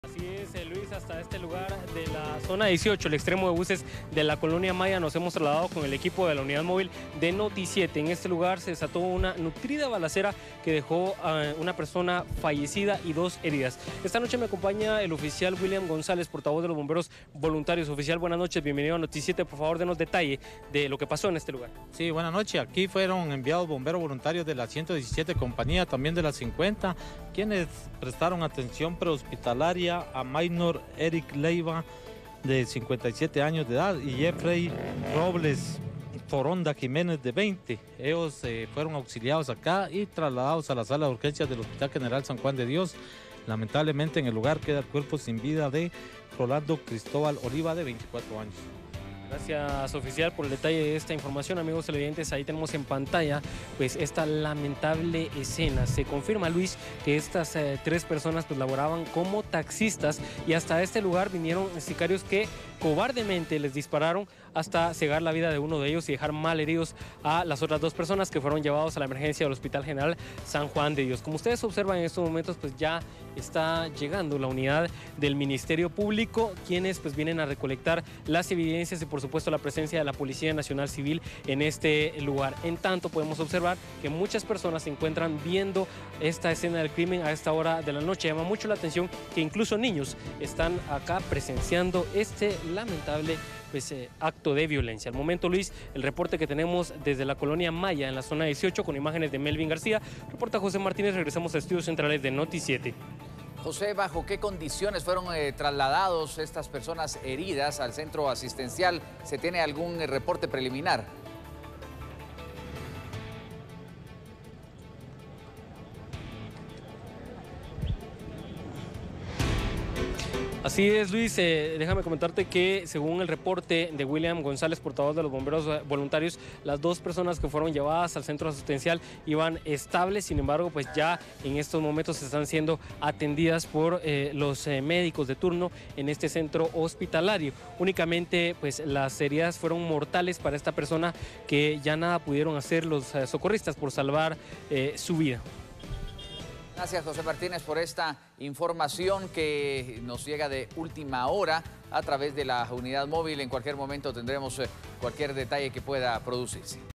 Ataque armado en Zona 18 deja un taxista fallecido
En el hecho se registraron dos personas heridas

Un ataque armado en la Zona 18 dejó como resultado una persona fallecida, de 24 años de edad, y dos heridas. Autoridades confirmaron que estas tres personas laboraban como taxistas.